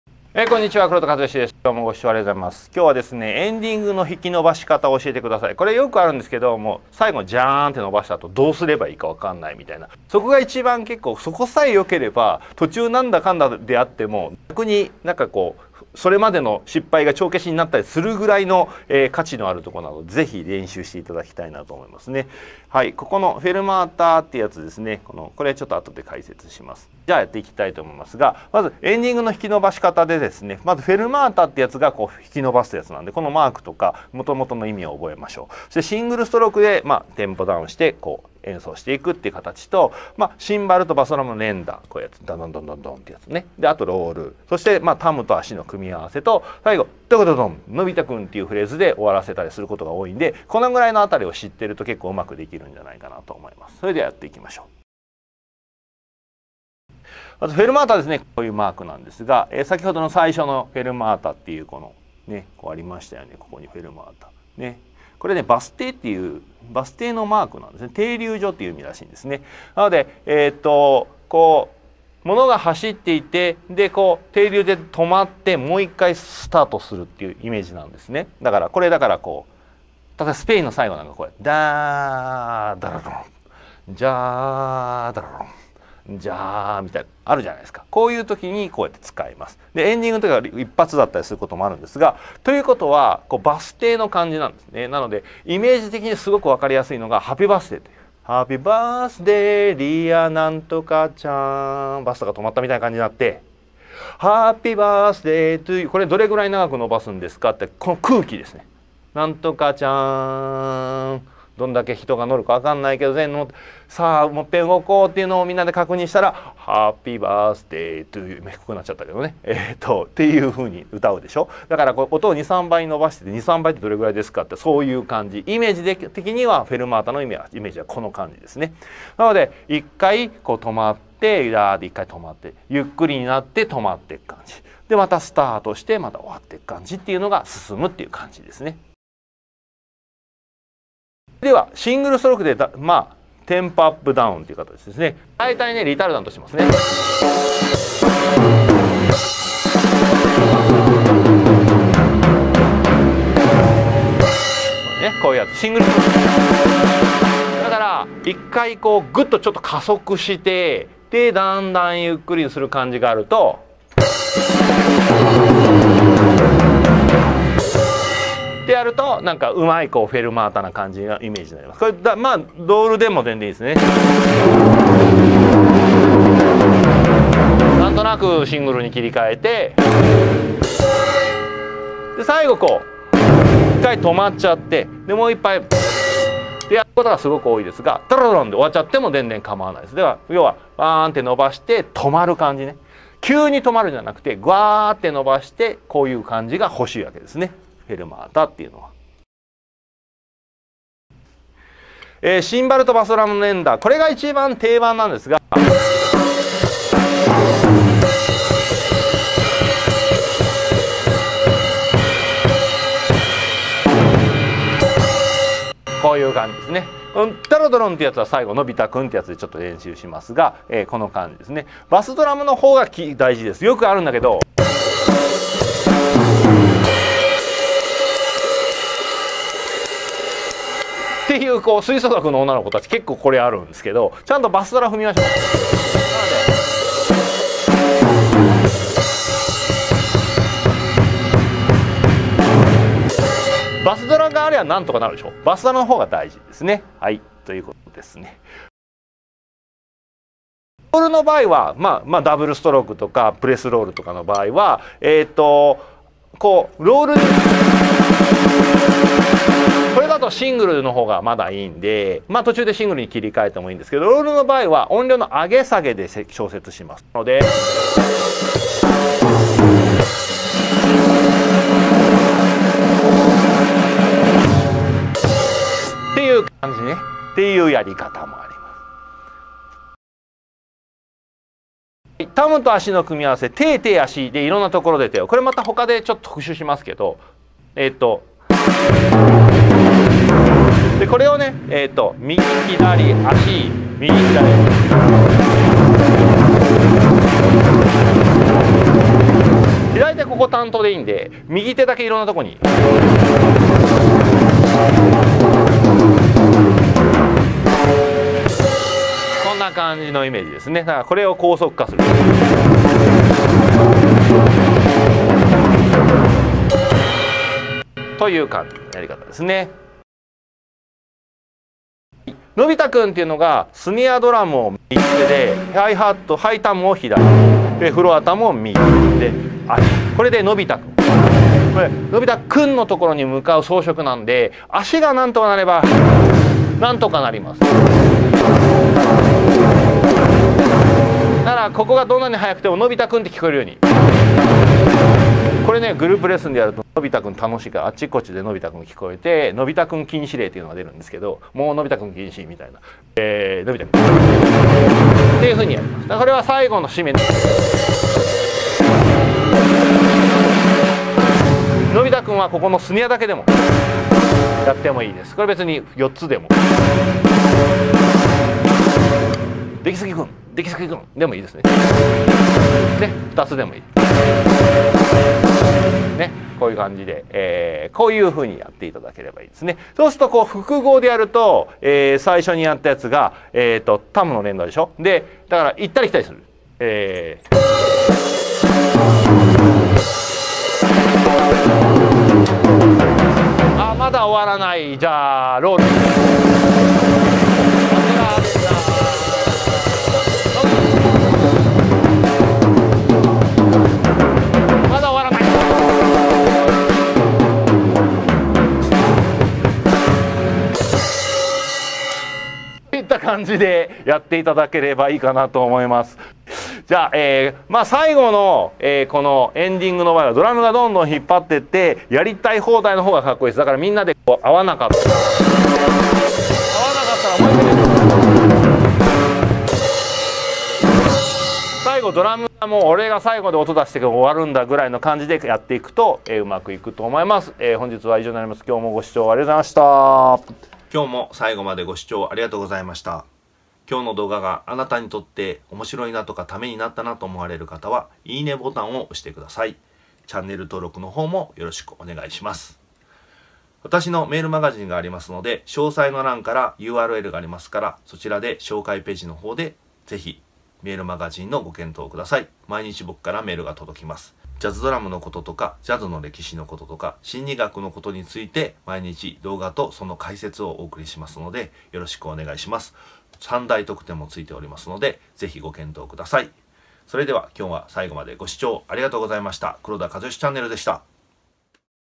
エンディング連打での引き伸ばし方！ファルマータって何？【ドラムのコツ】How to play the drum for the ending? And What is the Fermata?
​曲の最後はドラムの見せ場です
エンディングさえ決まればそれまでのミスも
帳消しくらい威力があります
是非マスターしましょう！

1. フェルマータ
2. シングルストロークでテンポアップ、ダウン
3. シンバルとバスドラム連打
4. ロール
5. タムと足の組み合わせ
6. のび太くんフレーズ

【参考動画】

【ドラムのコツ】
エンディング連打での引き伸ばし方！ファルマータって何？
How to play the drum for the ending? And What is the Fermata?